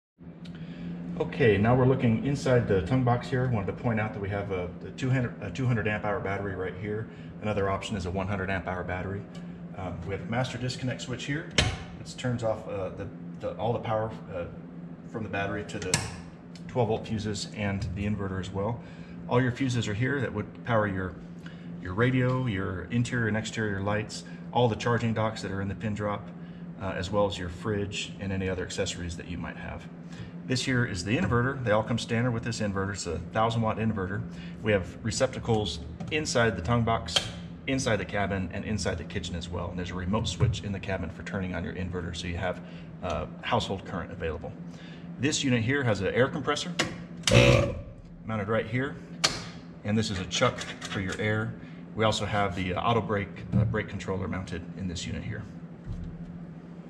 Micro Camper Tongue Box Storage and Electrical Accessories and Components - Pin Drop Travel Trailers
Located inside each Pin Drop Tongue Box is a 100 amp hour or 200 amp hour battery bank, a 1,000 watt inverter, 12 volt fuses, battery fuse and main disconnect. There is also a 110 VAC receptacle, a battery tender port, a remote solar panel port, two cigarette lighter power ports, and shore power for the chest refrigerator (on select models). On select models, the tongue box also stores an on board air compressor with a top side mounted air chuck, and a trailer mounted brake controller.
Extra storage space is also available for tools, shovel, tent stakes, tire jack and other camping essentials.